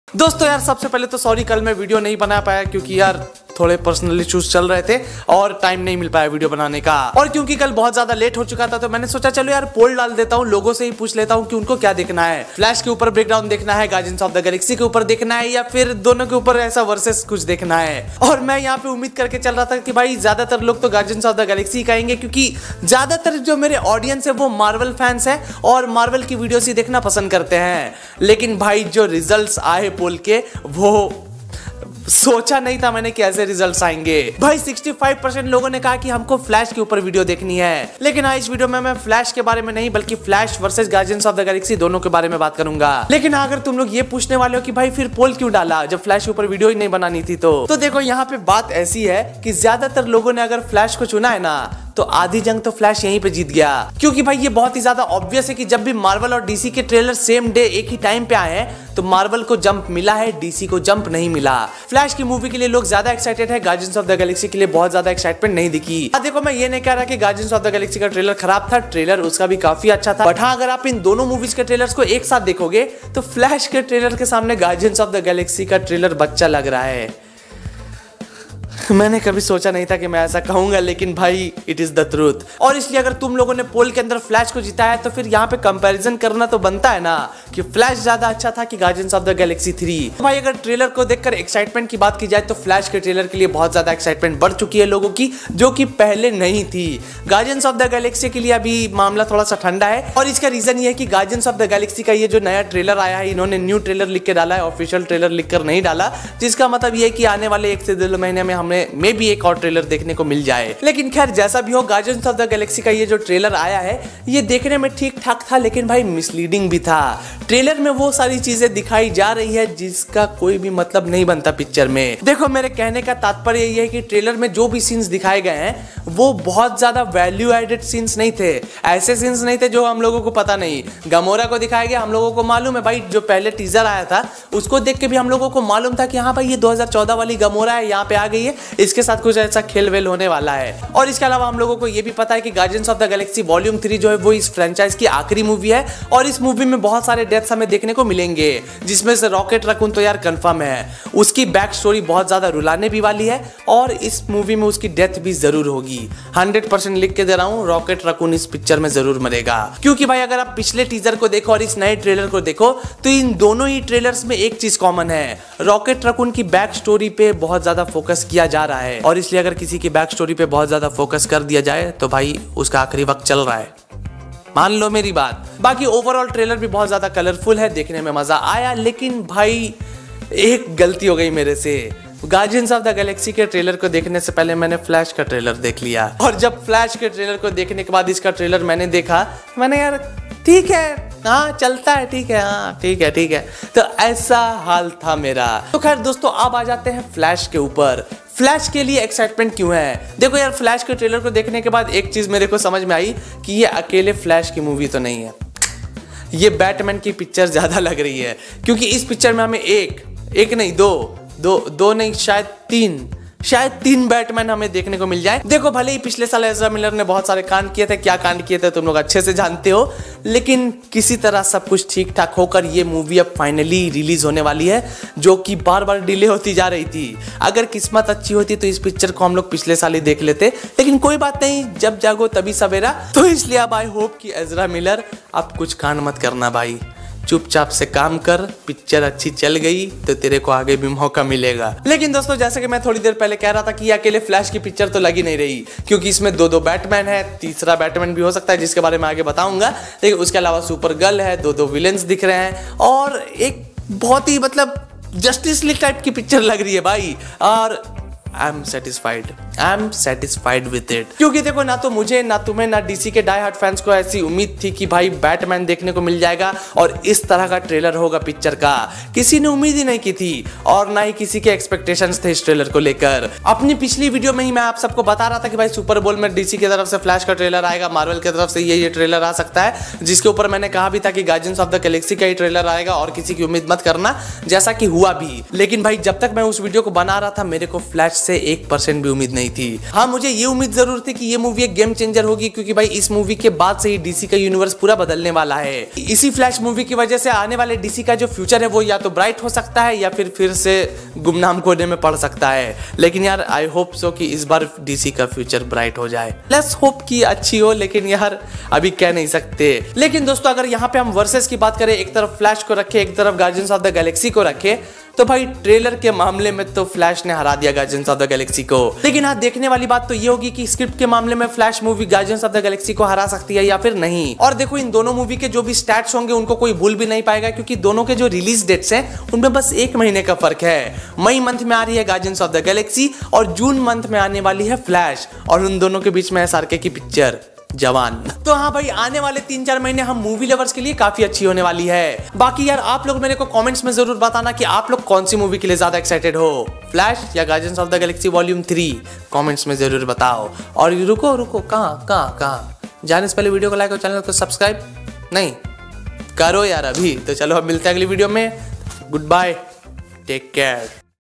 I'm Sorry, But Marvel Haar Gaya | The Flash Vs GOTG 3 Trailer Comparison | Filmy ZN
ZN Speaks -

Umeed Mat Rakho, Super Bowl Is Another Shit Event : Super Bowl 2023 All Details & Timings | Filmy ZN

Hello friends welcome to filmy zn youtube channel. In this video we will talk on the flash trailer and guardians of the galaxy trailer. Both trailers were awesome but if we compare both of them, then definitely flash will get more hype and likes against gotg.